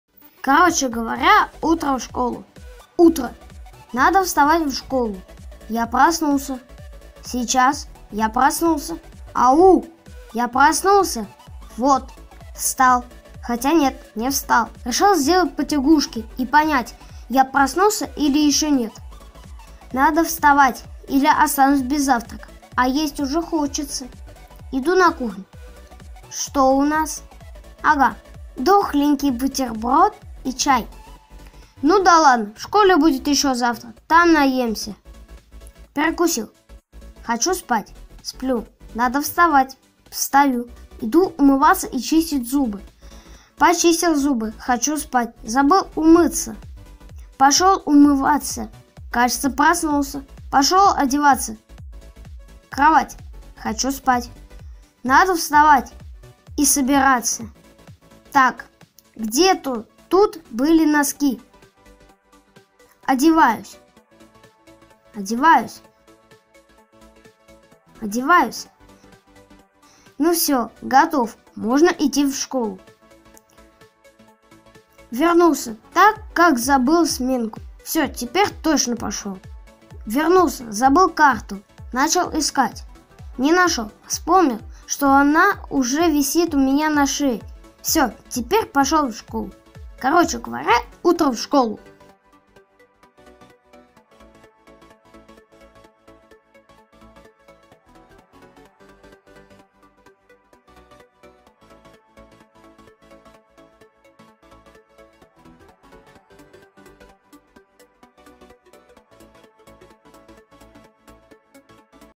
Короче говоря утро в школу / In short, morning to school
Развлекательное и познавательное видео для детей и взрослых, создано в юмористическом стиле.
Пишите в комментарии какое видео снять.